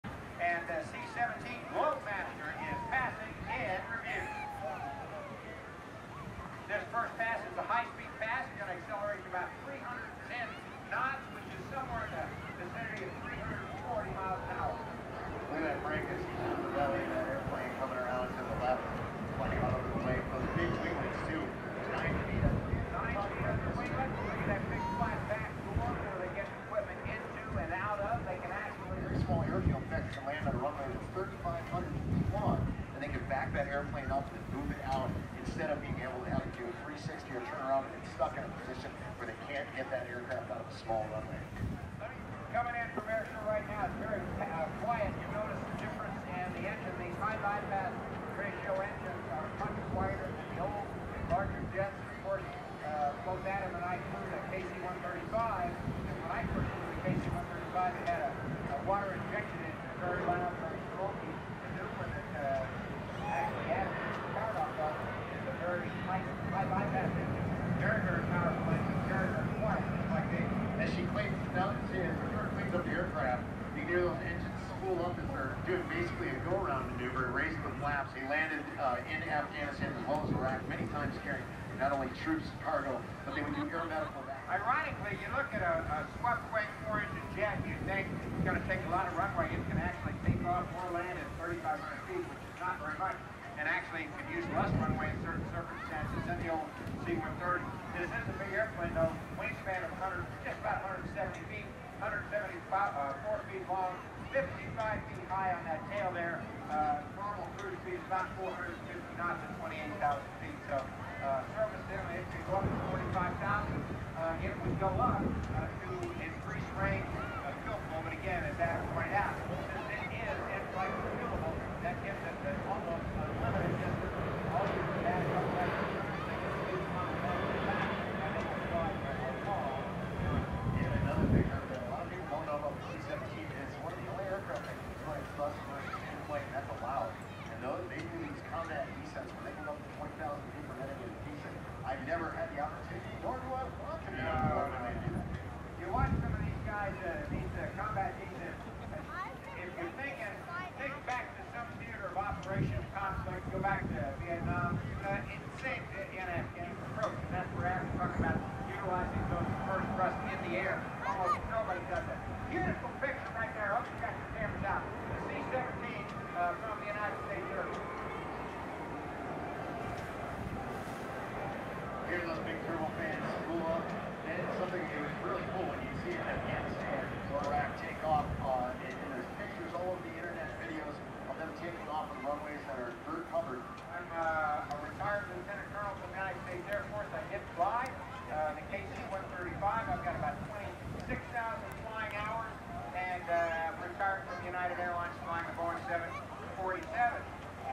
USAF C-17 Globemaster III Demo | Milwaukee Air & Water Show 2024
Big but surprisingly quiet boy.

Filmed with a Panasonic HC-WXF1 Camcorder.